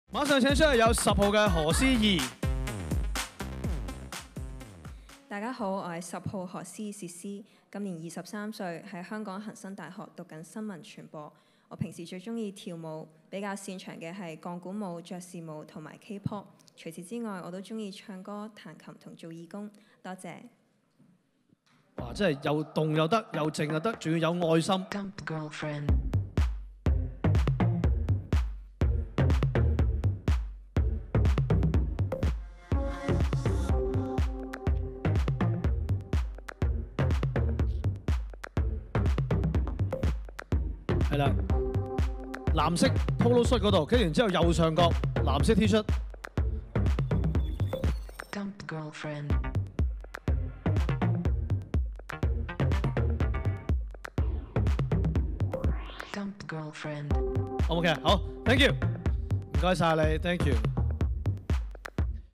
2022香港小姐競選｜入圍佳麗逐個睇 ｜10號｜何思懿｜港姐｜選美｜MissHK
【 2022香港小姐競選 】入圍佳麗逐個睇 10號 何思懿
《 2022香港小姐競選 》一眾入圍佳麗會晤傳媒，獲得屬於自己嘅號碼，開始屬於佢哋嘅逐夢之旅！一齊睇睇今屆參選嘅一眾佳麗啦！
10
佳麗姓名：何思懿 (Ceci Ho)
年齡：23
職業：學生
學歷：大學
身高(厘米)： 166.5
體重(公斤) ：53
嗜好：跳舞、唱歌、畫畫、鋼琴、義工服務
志向：成為一個有影響力的藝人。

Like + Follow = 緊貼 TVB 各劇集及藝員動態！